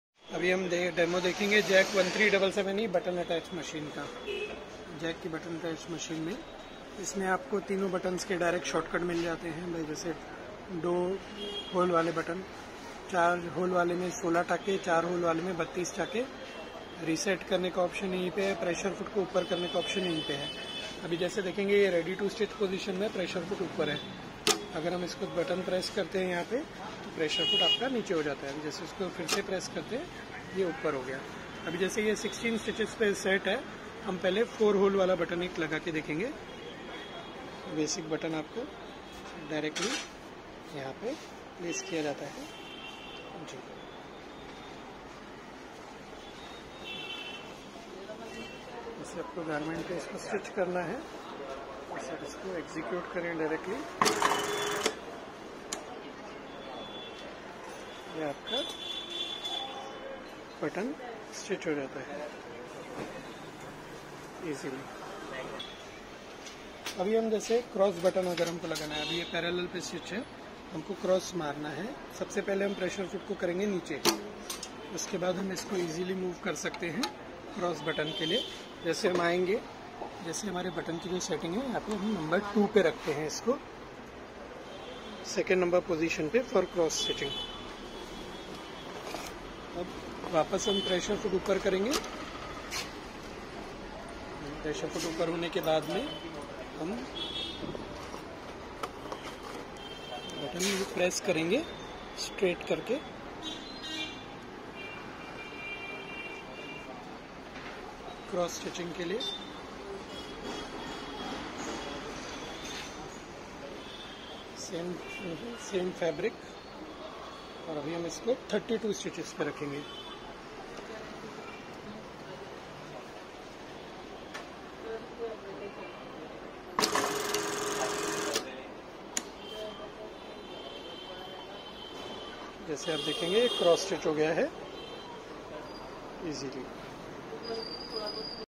Jack 1377E Demo
THIS IS A VIDEO DEMO FOR JACK 1377EB Button Attach Sewing Machine.
This shows the complete functionality of how a button is attached.